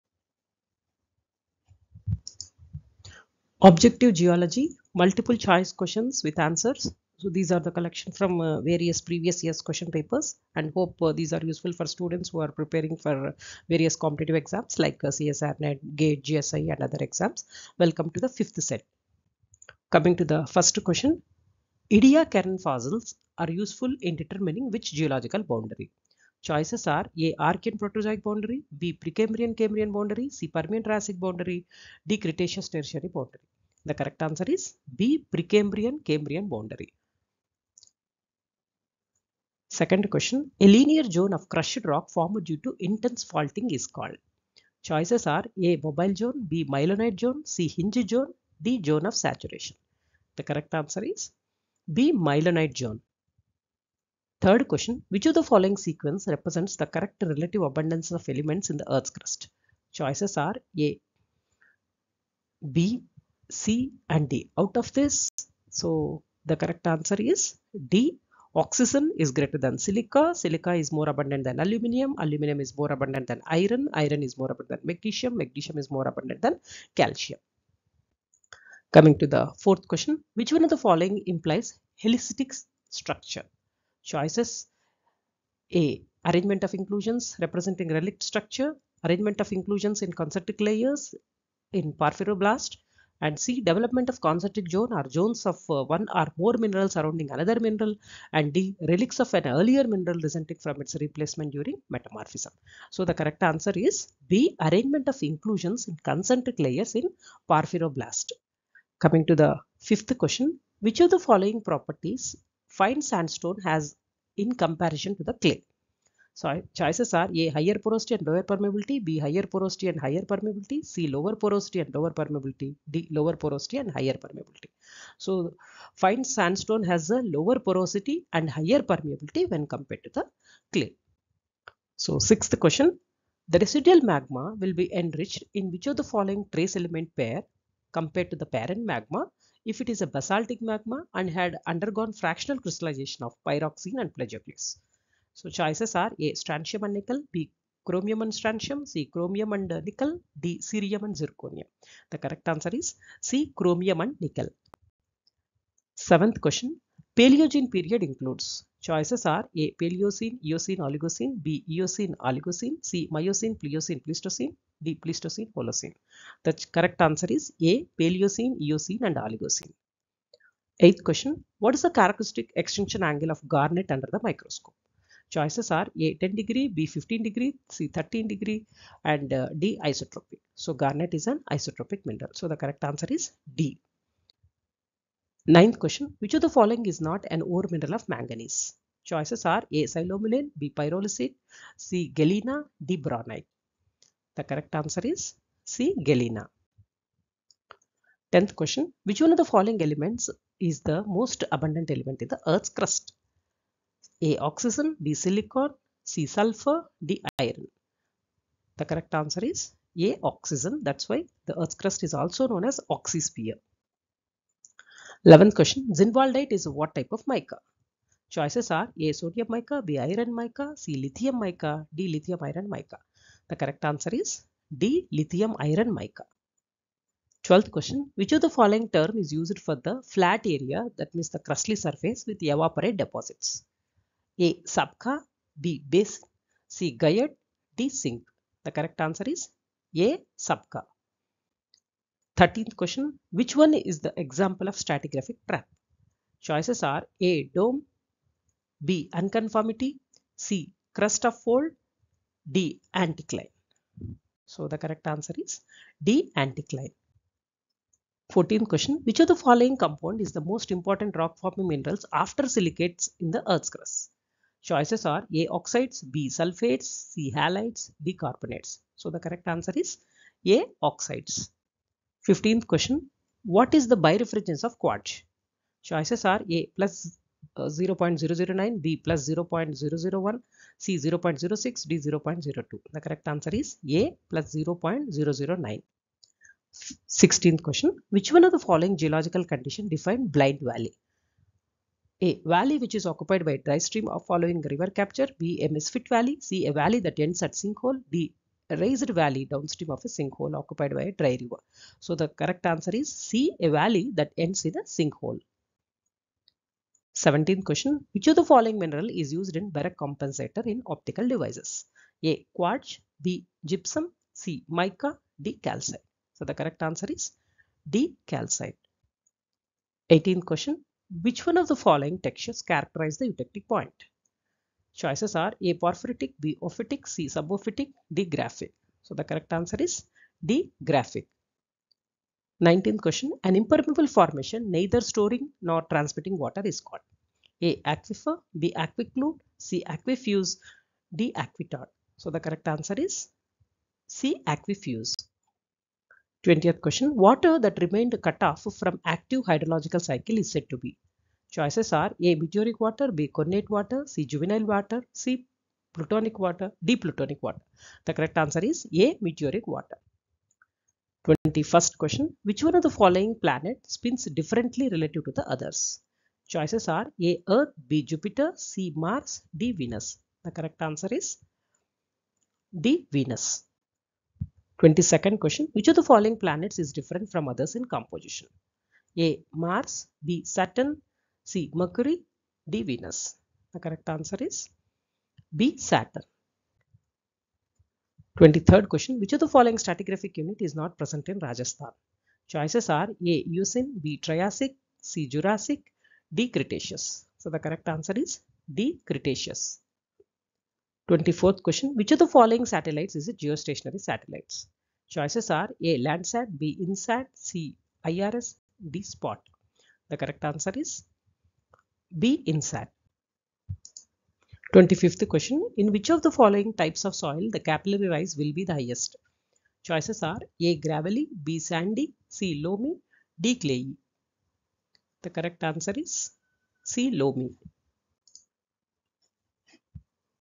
MCQ Ob Geology Set 5
You can practice your Geology  knowledge by using the MCQs provided here. These are the solved questions from various previous year Question papers of UPSC, other Geology related exams. These questions can be very helpful for preparing for all of the geology-related competitive exams, including the Technical Assistants Mines and Geology, UPSC Geologists, Hydrogeologists, APSET exams, as well as other competitive exams and University M.Sc entrance tests. These were created after reviewing numerous previous question papers from past exams.